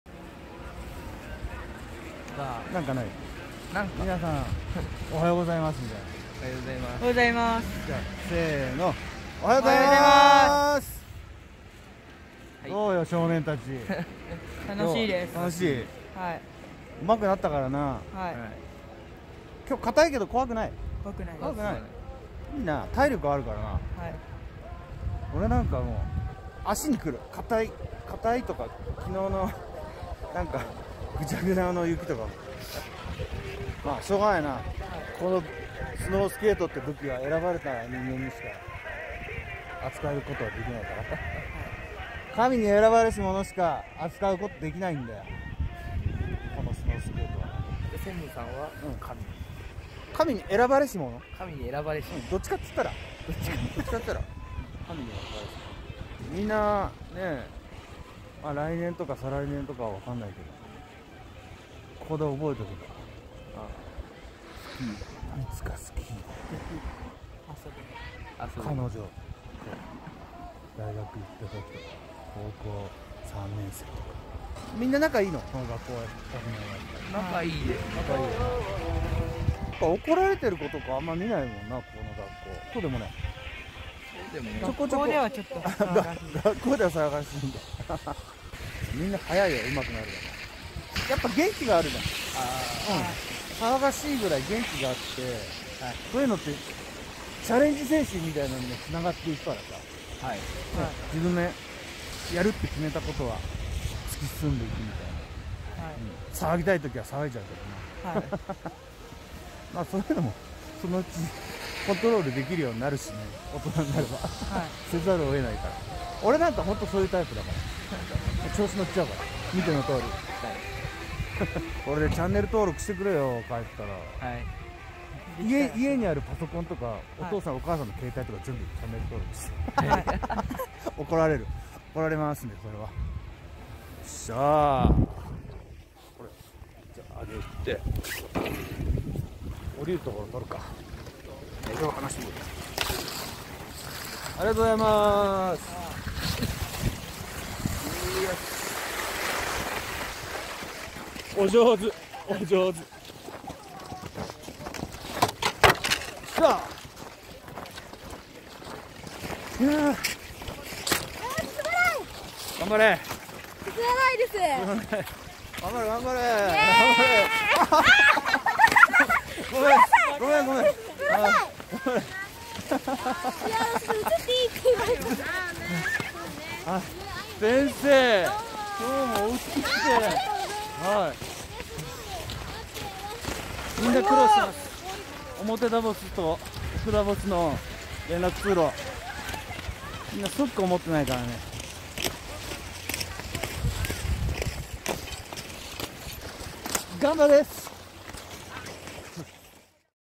吹上北中学校の男子リフトと女子クラッシュ in 菅平高原スキー教室
総集編には入り切らなくてカットしてしまった動画。
もったいないのでｗ
テロップつけるトレーニングも兼ねて。